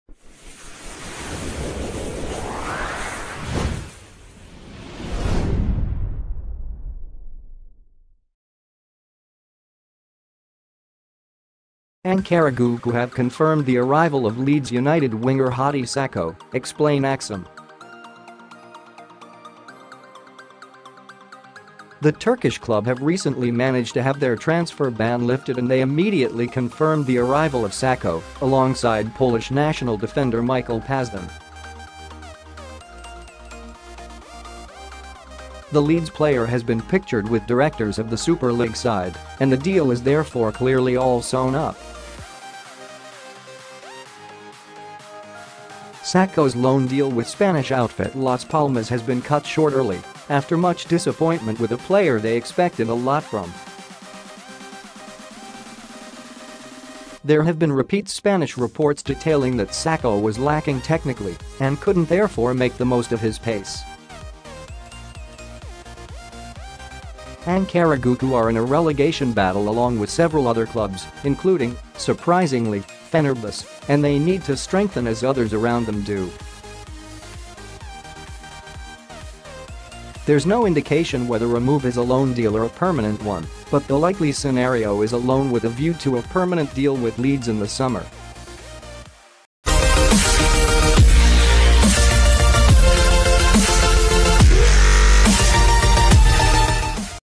Leeds United player pictured with directors of other club, transfer is all sewn up - Sport Witness
Ankaragucu have confirmed the arrival of Leeds United winger Hadi Sacko, explain Aksam.   The Turkish club have recently managed to have their transfer ban lifted and they immediately confirmed the arrival of Sacko, alongside Polish national defender Michael Pazdan.  The Leeds player has been pictured with directors of the Super Lig side, and the deal is therefore clearly all sewn up.  Sacko’s loan deal with Spanish outfit Las Palmas has been cut short early, after much disappointment with a player they expected a lot from. There have been repeat Spanish reports detailing that Sacko was lacking technically, and couldn’t therefore make the most of his pace.    Ankaragucu are in a relegation battle along with several other clubs, including, surprisingly, Fenerbahce, and they need to strengthen as others around them do.  There’s no indication whether a move is a loan deal or a permanent one, but the likely scenario is a loan with a view to a permanent deal with Leeds in the summer.